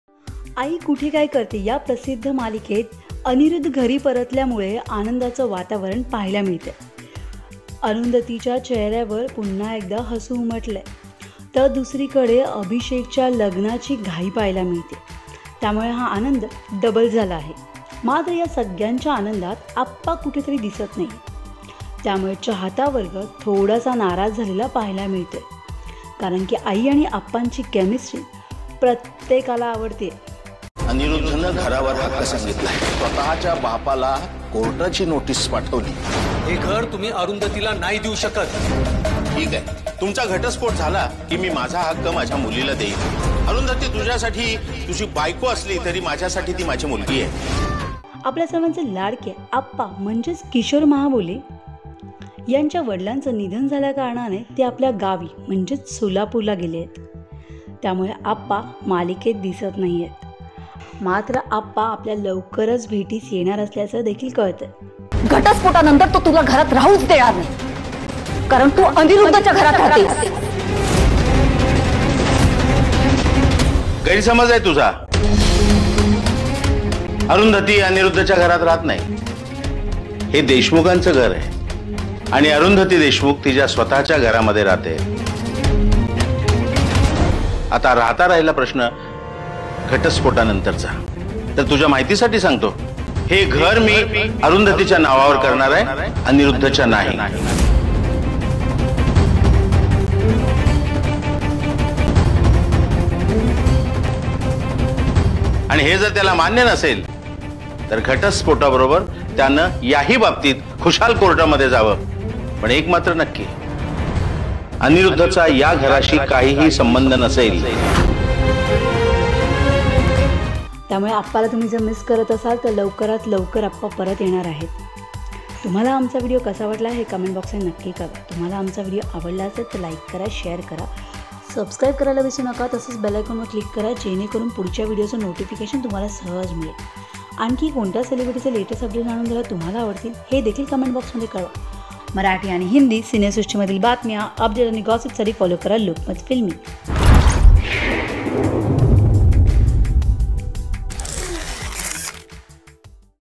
Aai Kuthe Kay Karte मालिकेतील आप्पा कुठे गेले? Aappa - Kishor Mahabole | Lokmat Filmy
आई कुठे काय करते या प्रसिद्ध मालिकेत अनिरूद्ध घरी परल्यामूळे आंनदाच वातावरण पाहिला मिळतय अरूधंतीच्या चेह-यावर पुन्हा एकदा हसू उमटलं आहे...तर दुसरीकडे अभिषेकची लगीन घाई पाहिला मिळतेय त्यामुळे हा आंनद डबल झाला आहे ... मात्र या सगळ्या आंनदात आप्पा कुठे दिसत असल्यामूळे चाहता वर्ग थोडासा नाराज झालेला पाहिला मिळतोय. मात्र आपल्या सर्वांचे लाडके आप्पा म्हणजेच किशोर माहाबोले यांच्या वडिलांच निधन झाल्या कारणाने त्यांना आपल्या गावी म्हणजेच सोलापूरला जाव लागलं.